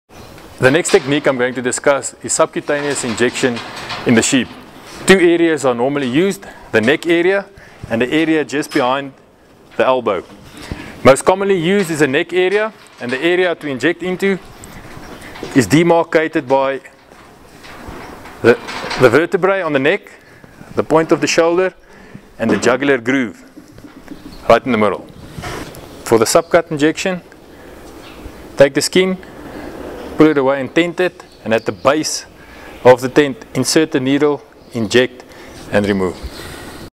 Sheep Subcutaneous Injection technique
Full demonstration on the correct technique of administering subcutaneous injections for sheep